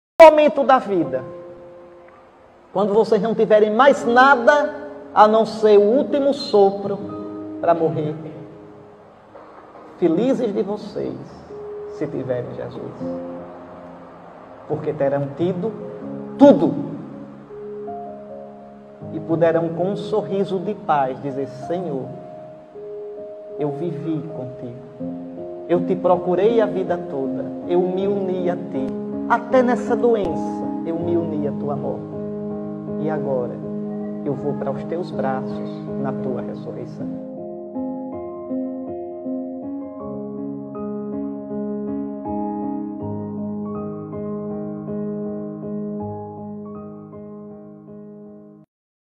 Viver bem é morrer bem.
Fragmento de uma palestra de Dom Henrique Soares da Costa.